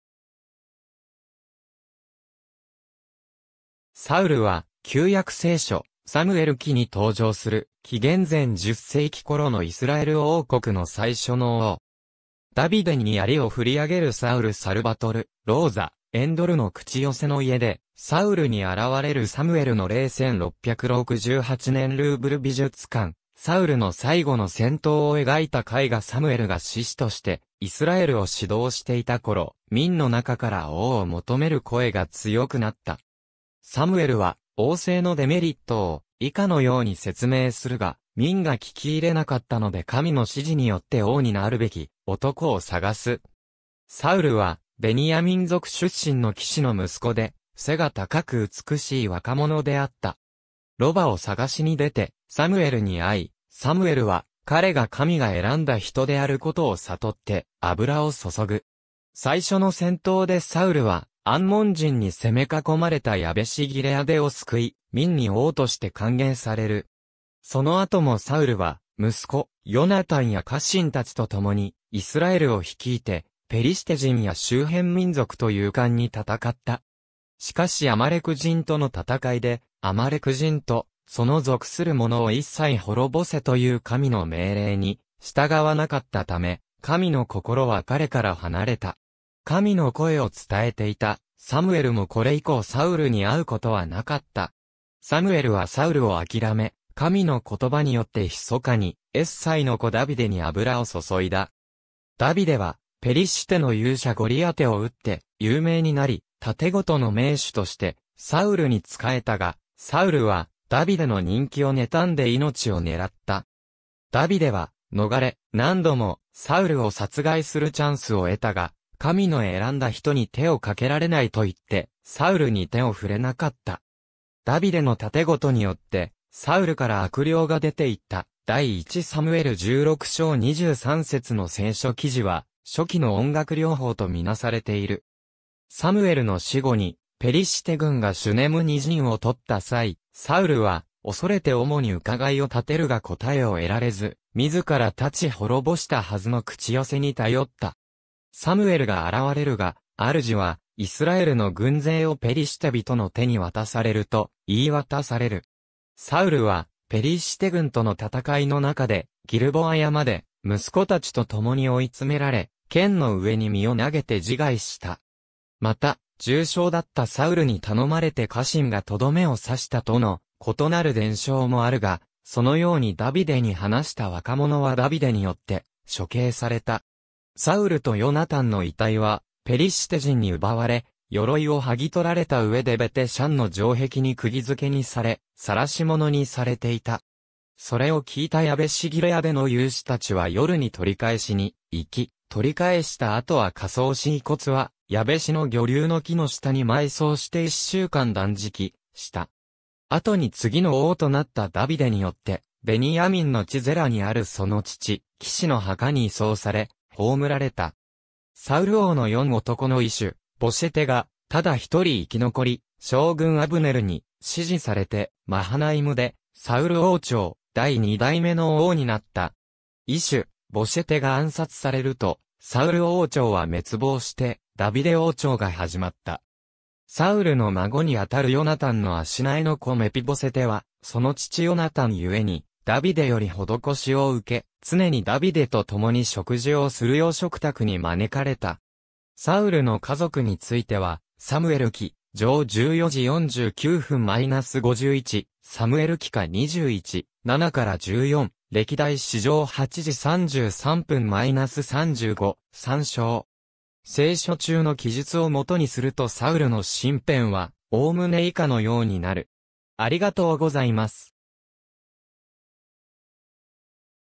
サウル
古代イスラエル・ユダの君主
サウル（Saul、ヘブライ語: שָׁאוּל‎、Šāʾūl）は、旧約聖書『サムエル記』に登場する、紀元前10世紀頃のイスラエル王国の最初の王。
ダビデに槍を振り上げるサウル サルヴァトル・ローザ『エン・ドルの口寄せの家でサウルに現れるサムエルの霊』1668年 ルーブル美術館 サウルの最期の戦闘を描いた絵画 サムエルが士師としてイスラエルを指導していた頃、民の中から王を求める声が強くなった。
サムエルは王政のデメリットを以下のように説明するが、民が聞き入れなかったので神の指示によって王になるべき男を捜す。
サウルはベニヤミン族出身のキシの息子で、背が高く美しい若者であった。
ロバを探しに出てサムエルに会い、サムエルは彼が神が選んだ人であることを悟って油を注ぐ。
最初の戦闘でサウルはアンモン人に攻め囲まれたヤベシ・ギレアデを救い、民に王として歓迎される。
その後もサウルは息子ヨナタンや家臣たちと共にイスラエルを率いて、ペリシテ人や周辺民族と勇敢に戦った。
しかしアマレク人との戦いで「アマレク人とその属するものを一切滅ぼせ」という神の命令に従わなかったため、神の心は彼から離れた。
神の声を伝えていたサムエルもこれ以降サウルに会うことはなかった。
サムエルはサウルをあきらめ、神の言葉によってひそかにエッサイの子ダビデに油を注いだ。
ダビデはペリシテの勇者ゴリアテを討って有名になり、竪琴の名手としてサウルに仕えたが、サウルはダビデの人気をねたんで命を狙った。
ダビデは逃れ、何度もサウルを殺害するチャンスを得たが、「神の選んだ人に手をかけられない」といってサウルに手を触れなかった。
ダビデの立琴によってサウルから悪霊が出て行った、第一サムエル16章23節の聖書記事は初期の音楽療法とみなされている。
サムエルの死後にペリシテ軍がシュネムに陣を取った際、サウルは恐れて主に伺いをたてるが答えを得られず、自ら断ち滅ぼしたはずの口寄せに頼った。
サムエルが現れるが「主はイスラエルの軍勢をペリシテびとの手に渡される」と言い渡される。
サウルはペリシテ軍との戦いの中で、ギルボア山で息子たちと共に追い詰められ、剣の上に身を投げて自害した。
また「重傷だったサウルに頼まれて家臣がとどめをさした」との異なる伝承もあるが、そのようにダビデに話した若者はダビデによって処刑された。
サウルとヨナタンの遺体はペリシテ人に奪われ、鎧を剥ぎ取られた上でベテ・シャンの城壁に釘付けにされ晒し者にされていた。
それを聞いたヤベシ・ギレアデの勇士たちは夜に取り返しに行き、取り返した後は火葬し遺骨はヤベシのギョリュウの木の下に埋葬して一週間断食した。
後に次の王となったダビデによって、ベニヤミンの地ゼラにあるその父キシの墓に移送され葬られた。
サウル王の四男のイシュ・ボシェテがただ一人生き残り、将軍アブネルに支持されて、マハナイムでサウル王朝第2代目の王になった。
イシュ・ボシェテが暗殺されるとサウル王朝は滅亡して、ダビデ王朝が始まった。
サウルの孫に当たるヨナタンの足萎えの子メピボセテは、その父ヨナタンゆえにダビデより施しを受け、常にダビデと共に食事をするよう食卓に招かれた。
サウルの家族については『サムエル記』上14:49-51、サムエル記下21:7-14、歴代誌上8:33-35参照。
聖書中の記述を元にするとサウルの身辺は概ね以下のようになる。